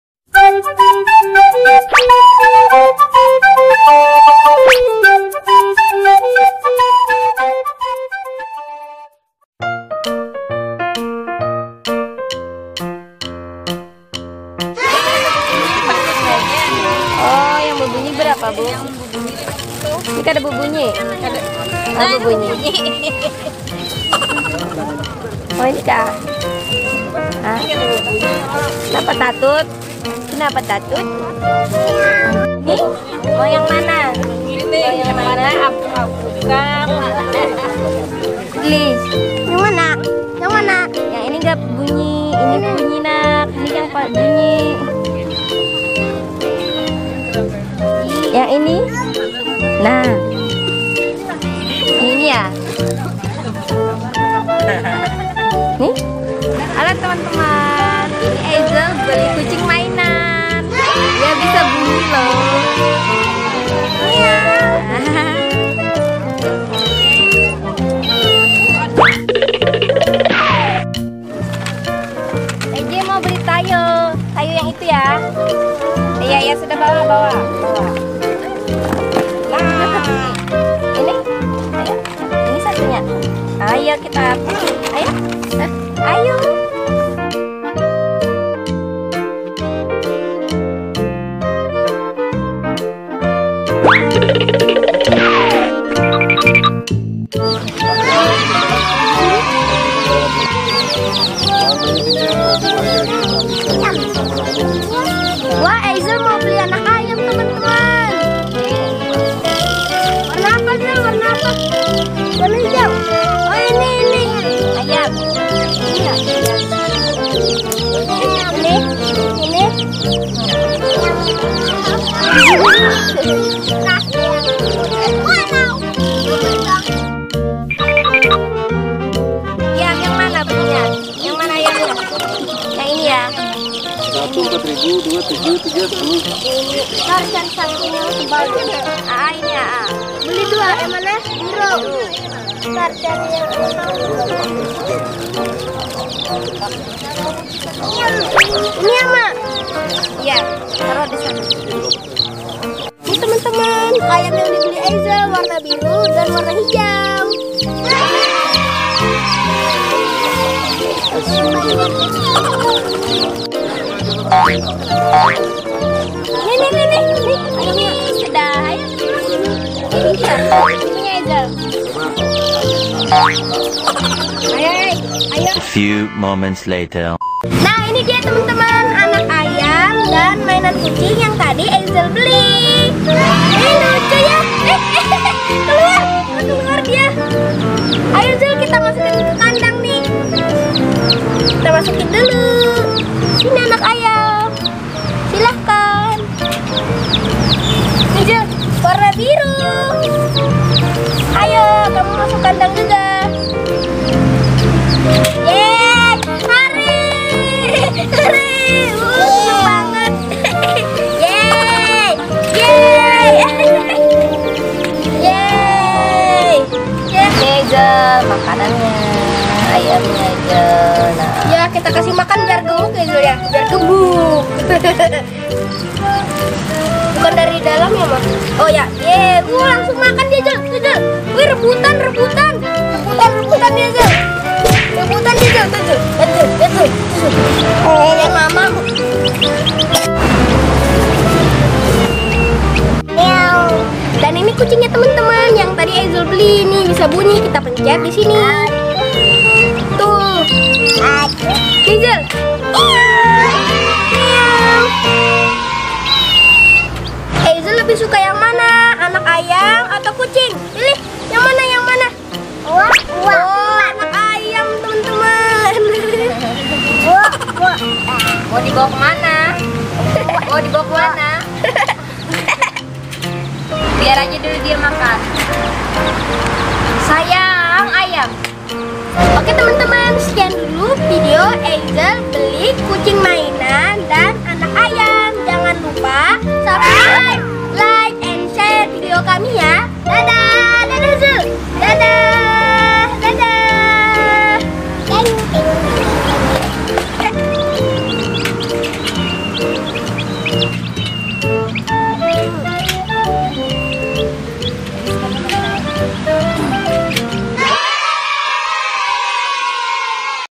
Anak Lucu Beli Anak Ayam Warna Warni
anak lucu ini beli anak ayam warna warni di pasar joging